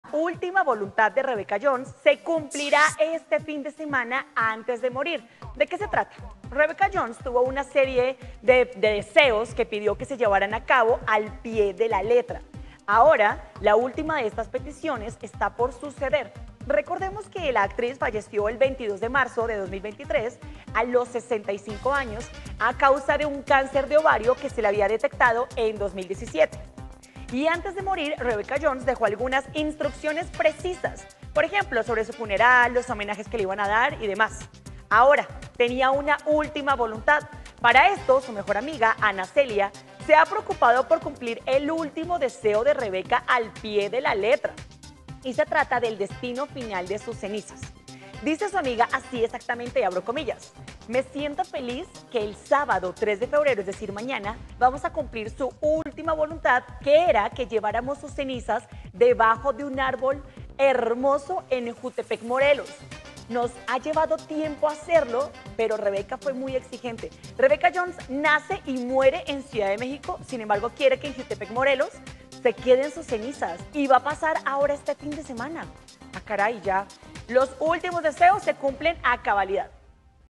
Rebeca Jones || Noticias con Juan Carlos Valerio
Entérate || Será cumplido el último deseo de Rebeca Jones, recordemos que ella falleció a causa de cáncer de ovario.

Entérate aquí cuál es ese último deseo. 👇🏻